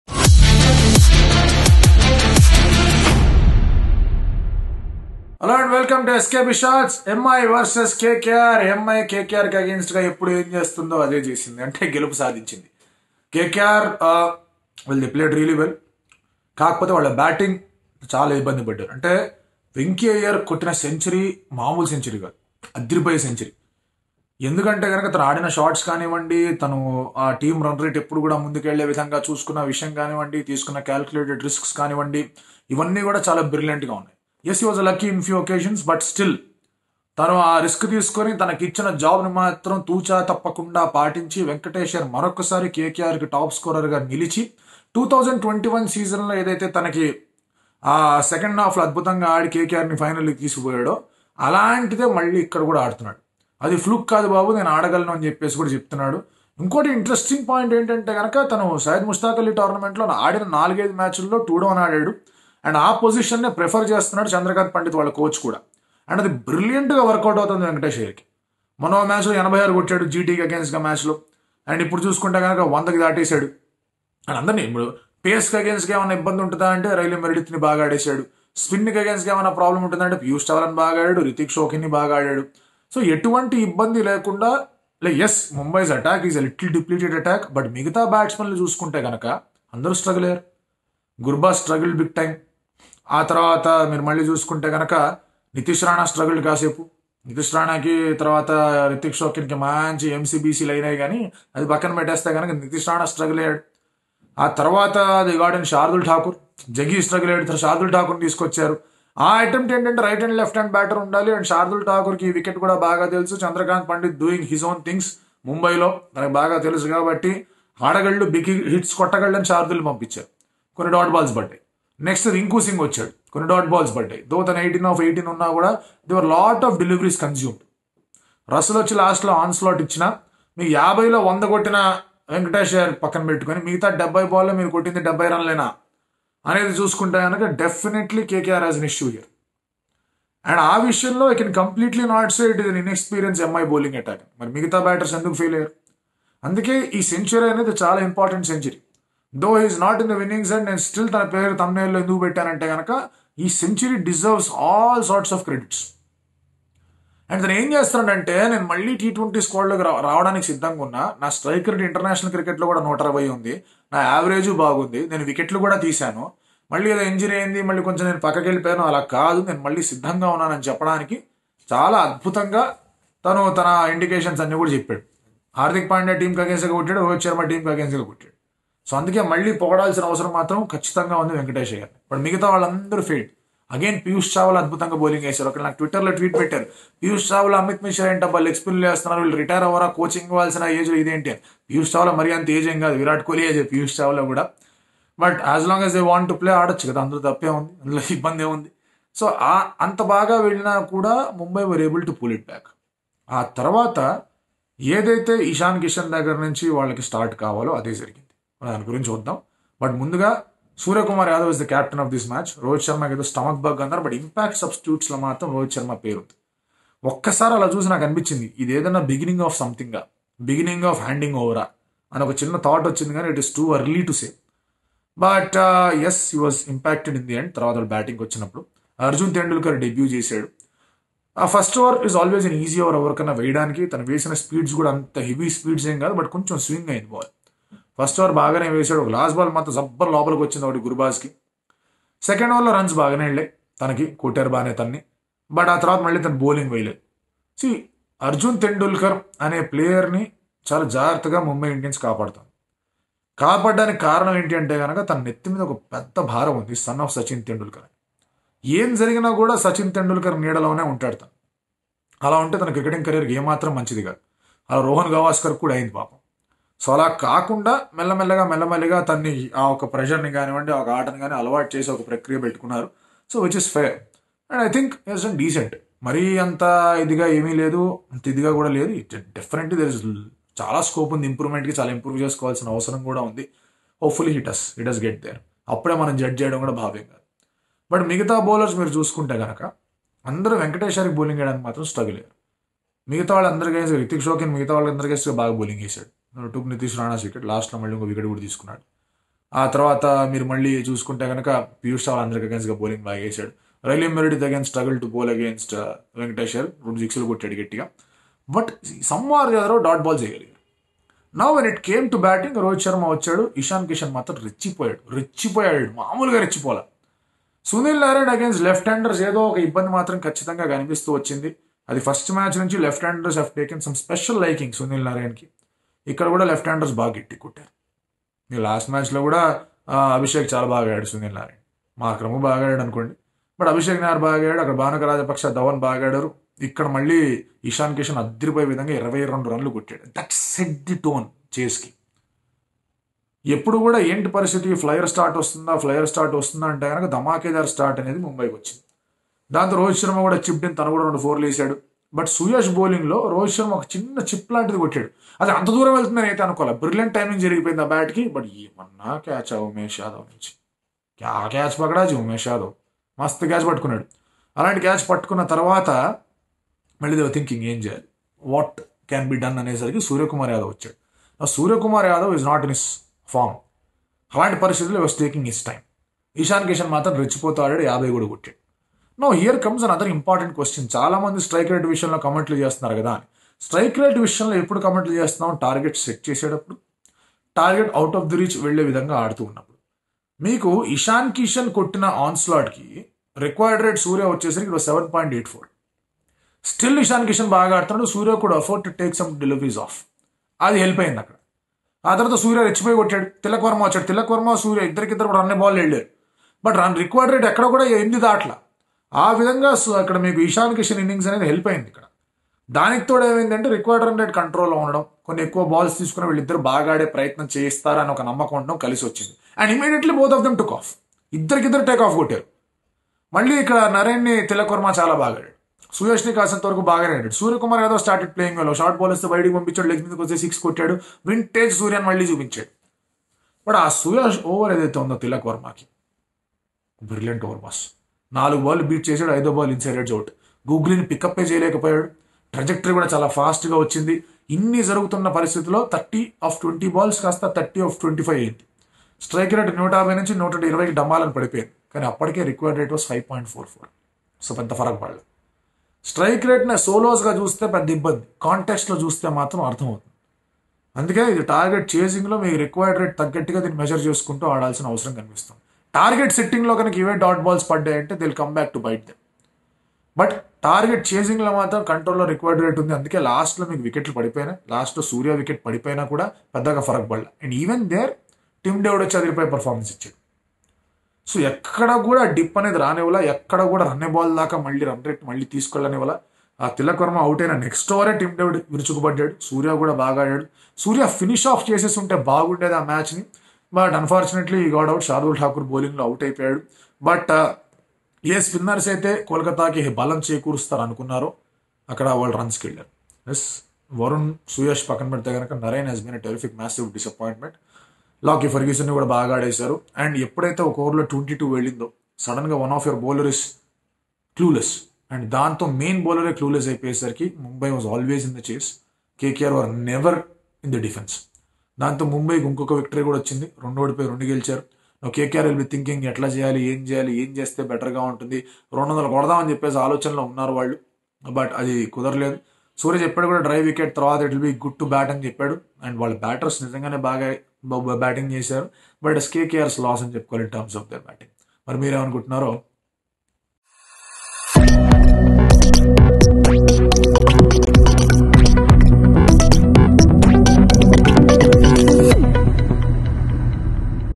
MI vs KKR Review | Venky Iyer 100 | Ishan Kishan 50 | IPL 2023
Watch Now - MI vs KKR Review | Venky Iyer 100 | Ishan Kishan 50 | IPL 2023